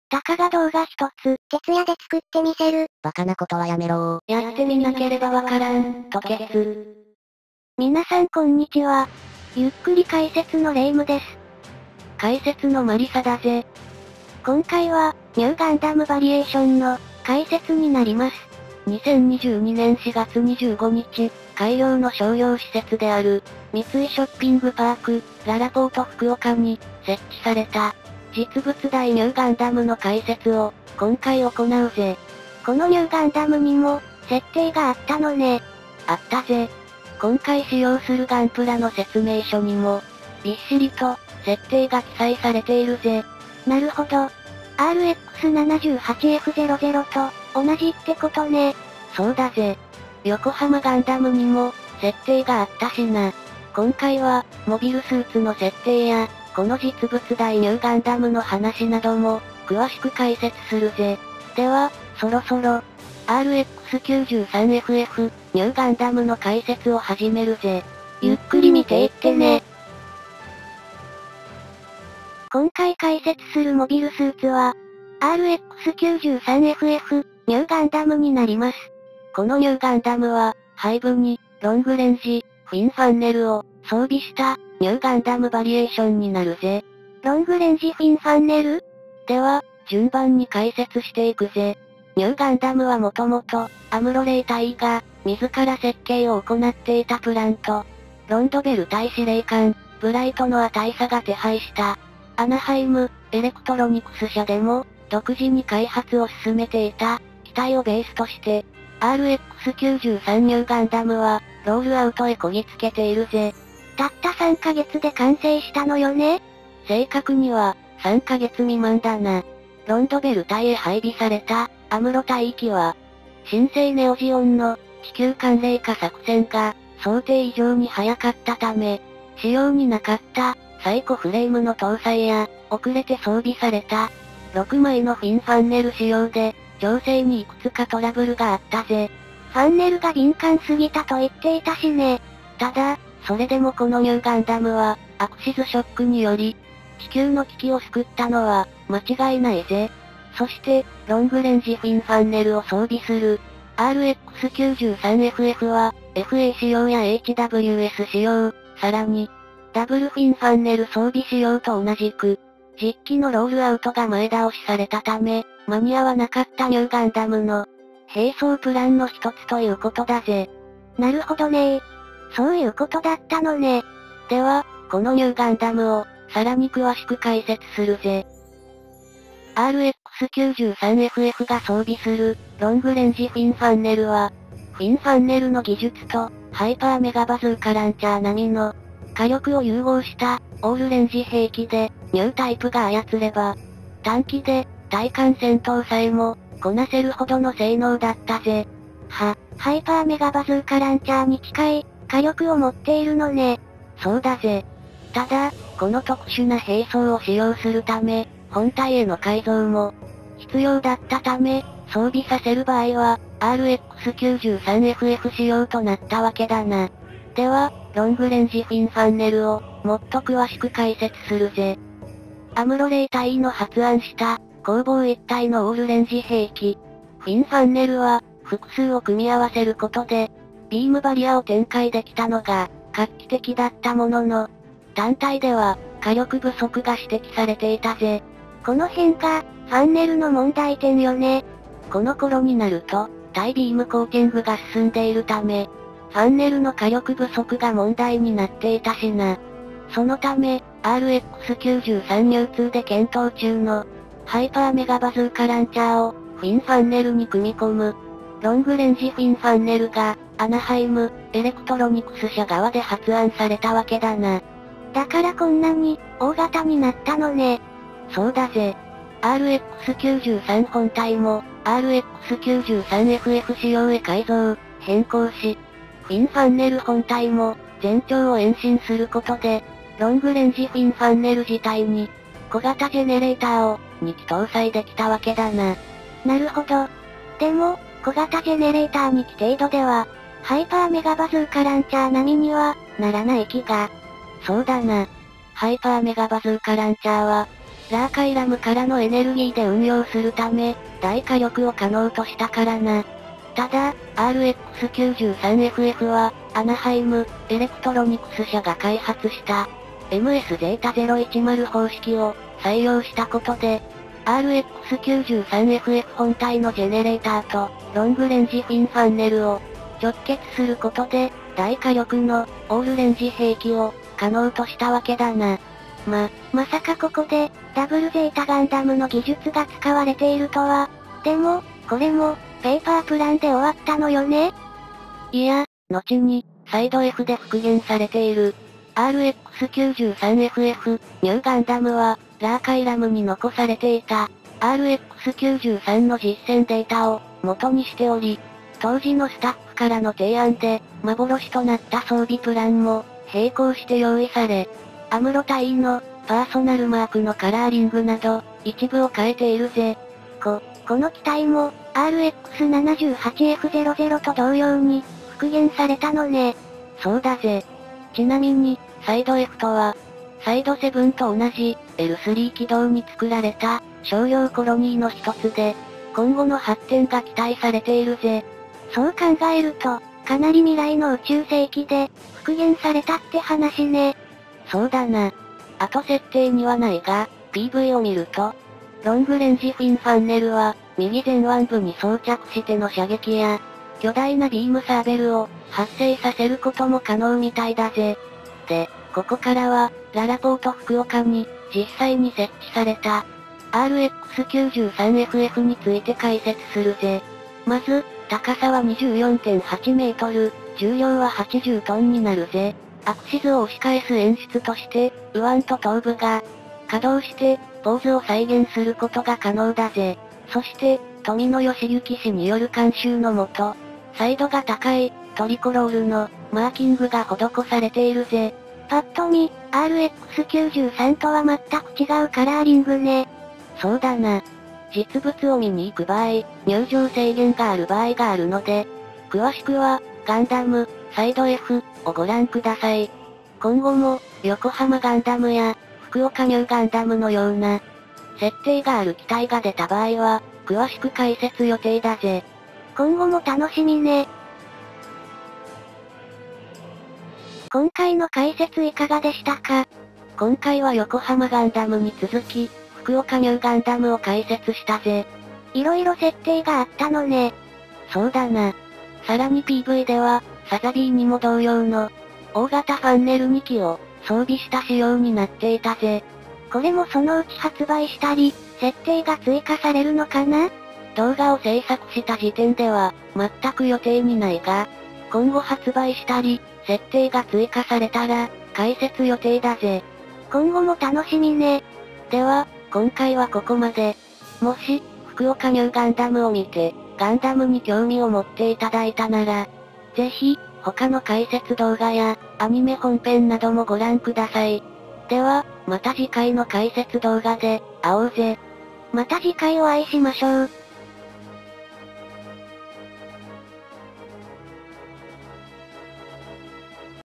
【福岡νガンダム】RX-93ff νガンダム 解説【ガンダム解説】part13【ロングレンジフィンファンネル】【逆襲のシャア】【アムロ・レイ】【ガンプラ】【ゆっくり解説】【ニューガンダム】ららぽーと
ゆっくり解説動画をご覧頂きありがとうございます。
ガンダム解説動画になります。

第13回　RX-93ff　νガンダム(福岡νガンダム)
ロングレンジ・フィン・ファンネル装備型
の解説になりますので温かい目でご覧ください

アムロ・レイが搭乗した機体とは別で
兵装プランのひとつらしい

チャンネル概要を読んでからご覧ください

ご意見ご感想は毎週行っている生配信へ